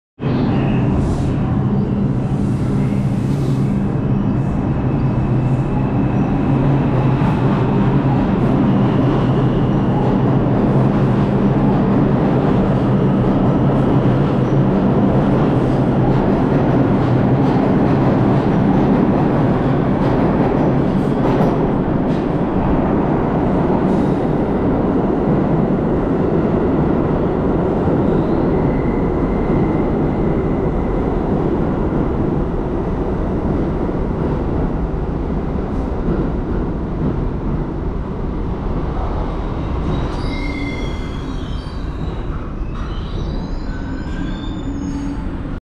4K 60FPS: R142 2 Train Leaving 72nd Street
Recorded with: GoPro Hero Black 11
and no this is not the only device I record with.